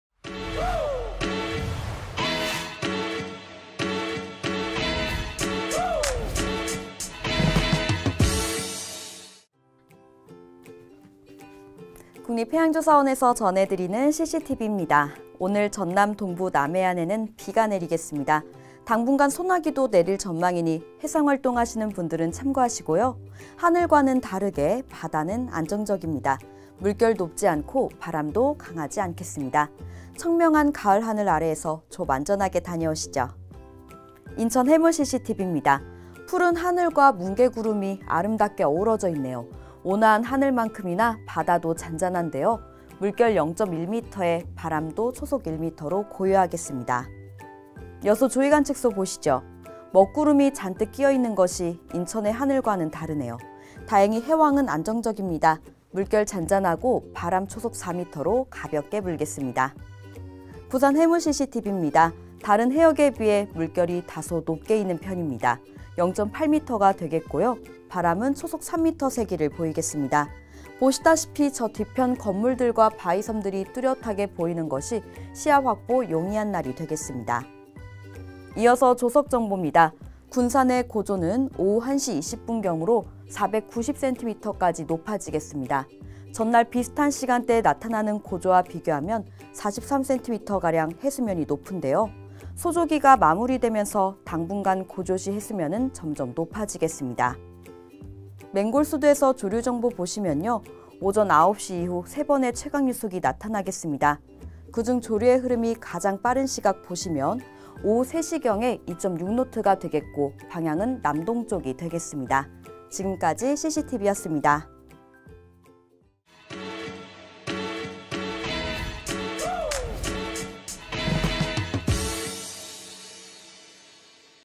[See Sea TV] 23년9월11일 청명한 가을하늘 아래 조업 안전히!/해양예보방송
국립해양조사원에서 전해드리는 see sea TV입니다.
오늘 전남동부 남해안에는 비가 내리겠습니다.
당분간 소나기도 내릴 전망이니 해상활동하시는 분들은 참고하시고요.
하늘과는 다르게 바다는 안정적입니다.
물결 높지 않고 바람도 강하지 않겠습니다.
청명한 가을하늘 아래에서 조업 안전하게 다녀오시죠.

인천 해무 CCTV입니다.
푸른 하늘과 뭉게구름이 아름답게 어우러져 있네요.
온화한 하늘만큼이나 바다도 잔잔한데요.
물결 0.1m에 바람도 초속 1m로 고요하겠습니다.

여수 조위관측소 보시죠.
먹구름이 잔뜩 끼어있는 것이 인천의 하늘과는 다르네요.
다행히 해황은 안정적입니다. 물결 잔잔하고 바람 초속 4m로 가볍게 불겠습니다.

부산 해무 CCTV입니다.
다른 해역에 비해 물결이 다소 높게 이는 편입니다.
0.8m가 되겠고요. 바람은 초속 3m 세기를 보이겠습니다.
보시다시피 저 뒤편 건물들과 바위섬들이 뚜렷하게 보이는 것이
시야확보 용이한 날이 되겠습니다.

이이서 조석 정보 입니다.
군산의 고조는 오후 1시 20분경으로 490cm까지 높아지겠습니다.
전날, 비슷한 시간 때 나타나는 고조와 비교하면 43cm가량 해수면이 높은데요.
소조기가 마무리되면서 당분간 고조 시 해수면은 점점 높아지겠습니다.

맹골수도에서 조류정보 보시면요.
오전 9시 이후 세 번의 최강유속이 나타나겠습니다.
그중 조류의 흐름이 가장 빠른 시각 보시면,
오후 3시경에 2.6노트가 되겠고 방향은 남동쪽이 되겠습니다.
지금까지 sstv였습니다.

- 남기연 해양캐스터